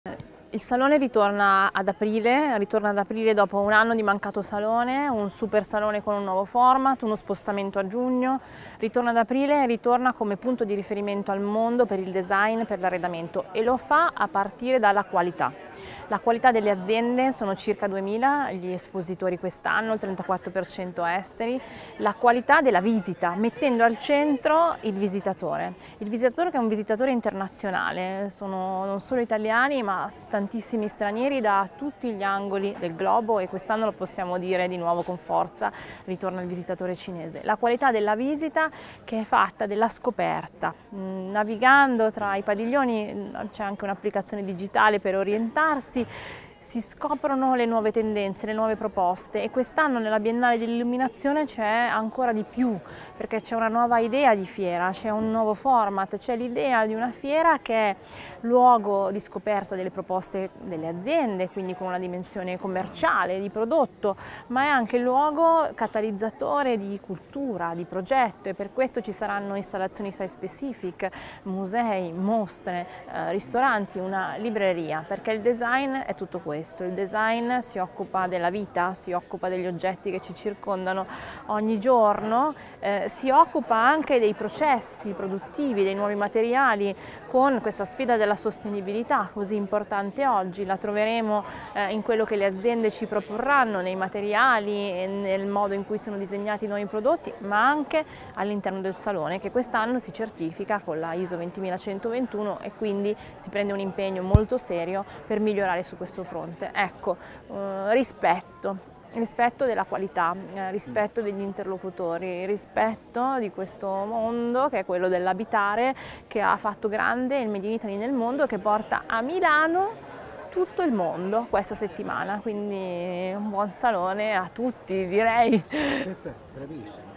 Maria Porro al Salone del Mobile 2023: la qualità premiata dai visitatori
Intervista a Maria Porro, presidente del Salone del Mobile.Milano
a cura di Sara Salmoiraghi